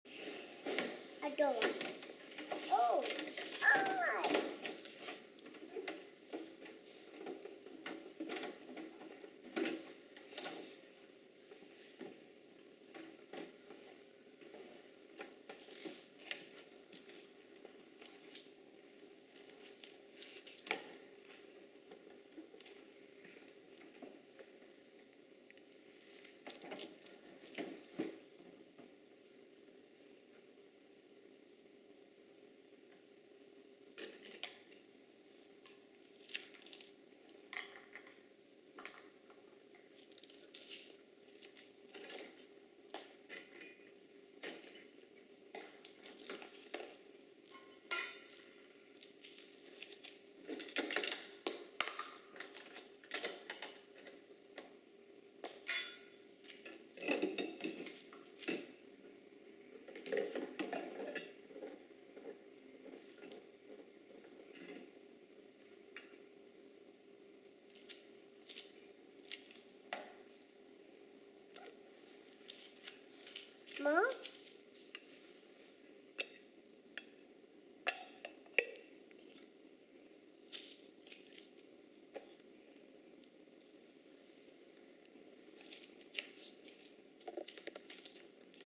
Girl And Her Kitchen
She is serving tea to her uncle.